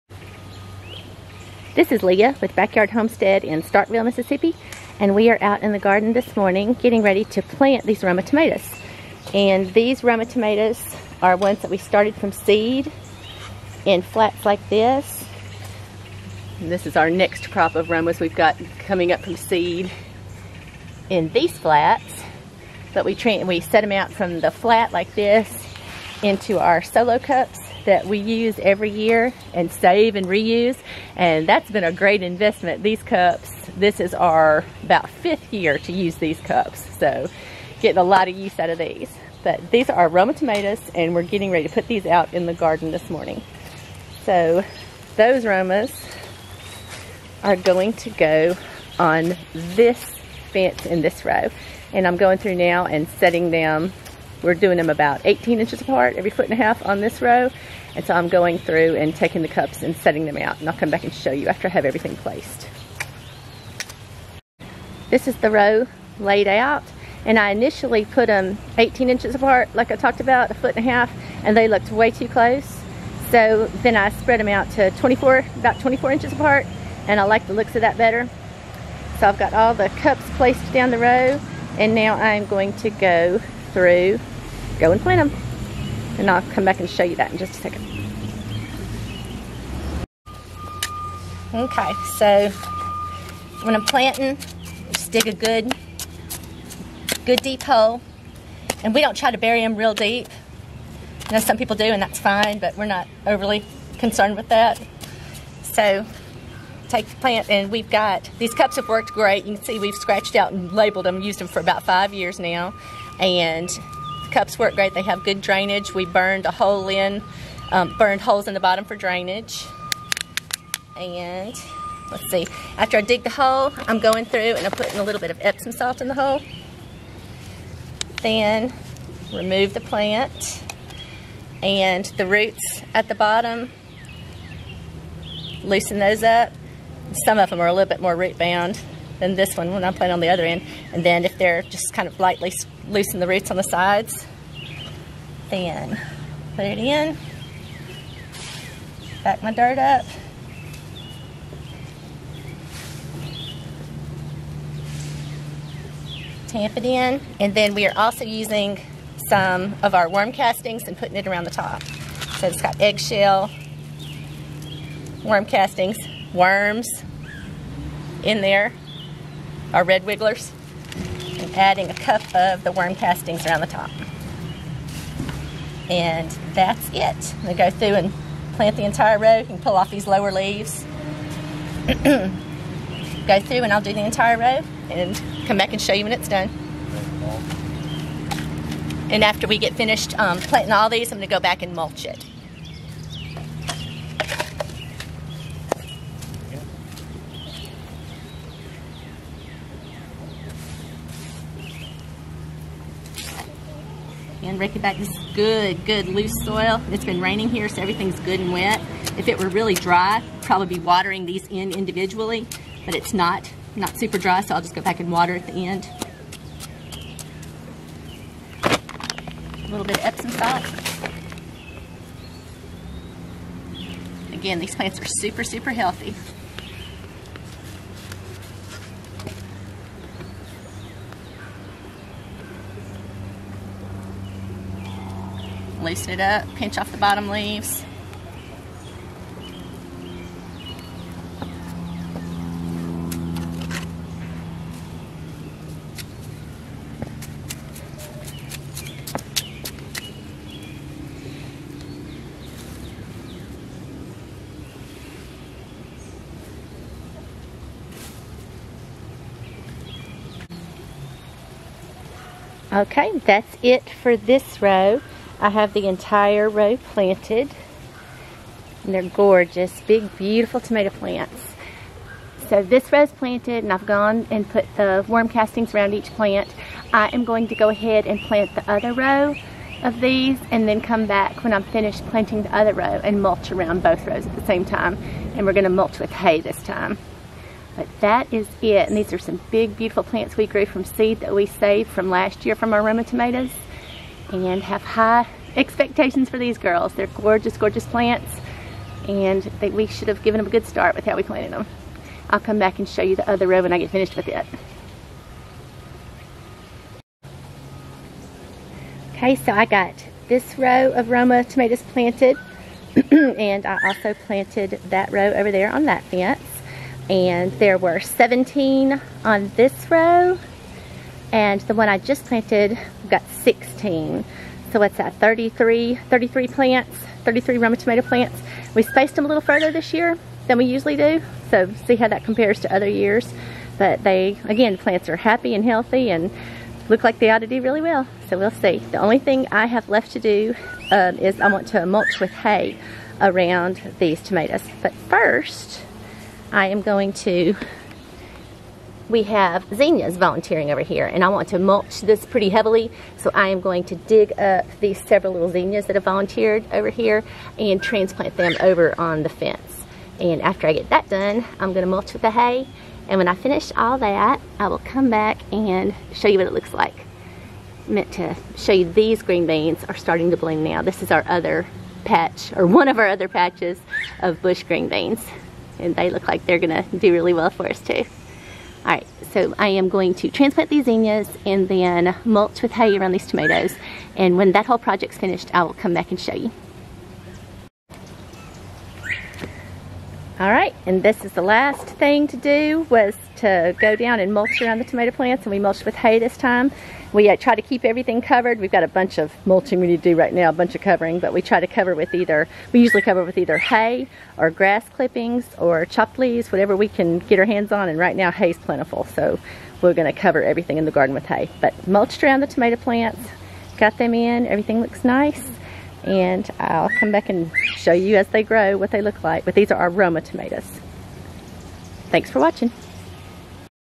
Planting Roma Tomatoes 2022
We started our Roma tomatoes out from seeds in seed flats. We then transferred the plants to larger solo cups. They are now large enough and the weather is right to plant them out into the garden.